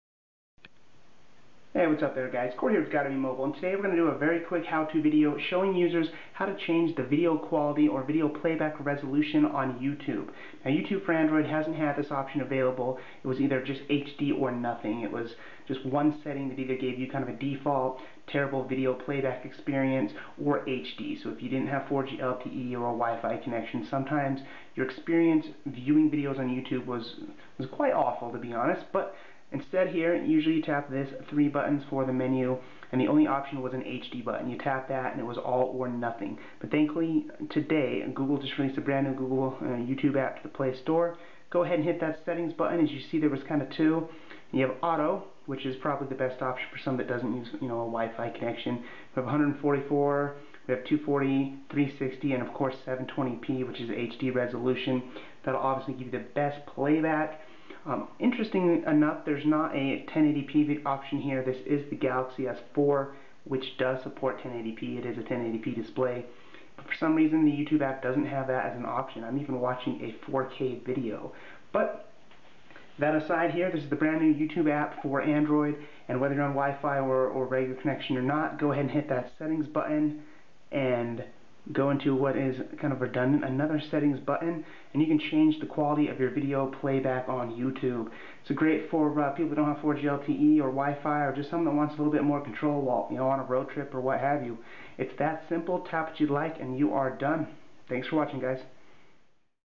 How to Change YouTube Playback Quality on Android
Here we quickly show how users can change the quality and resolution of YouTube video playback on Android devices. This is available on the new YouTube app from the Google Play Store. More at GottaBeMobile.com